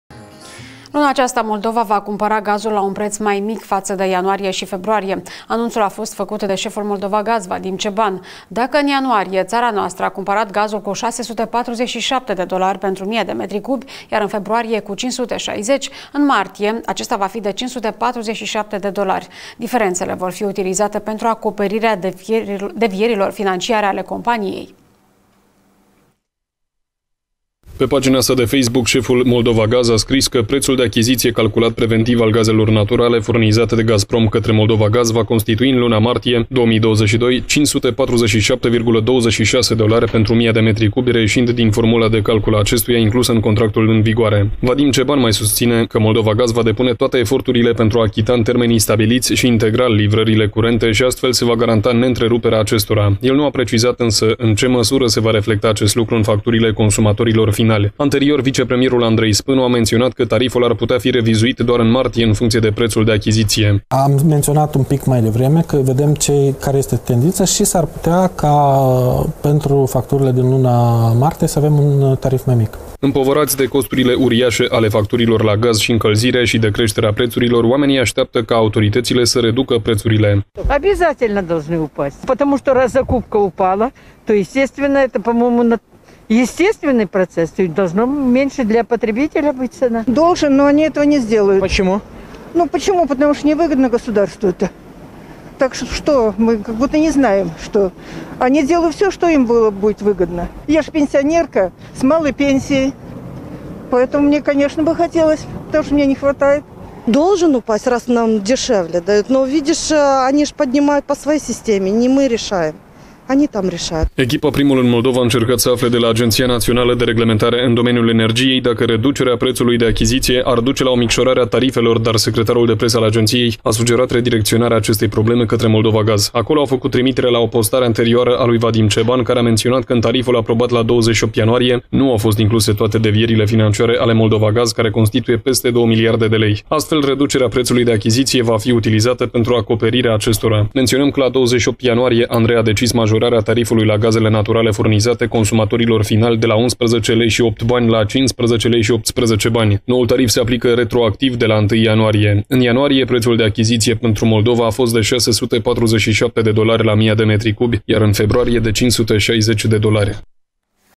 Preț mai mic pentru gaz în martie
Luna aceasta Moldova va cumpăra gazul la un preț mai mic față de ianuarie și februarie. Anunțul a fost făcut de șeful Moldovagaz, Vadim Ceban. Dacă în ianuarie țara noastră a cumpărat gazul cu 647 dolari pentru mia de metri cubi, iar în februarie- cu 560, în martie acesta va fi de 547 de dolari. Diferențele vor fi utilizate pentru acoperirea devierilor financiare ale companiei.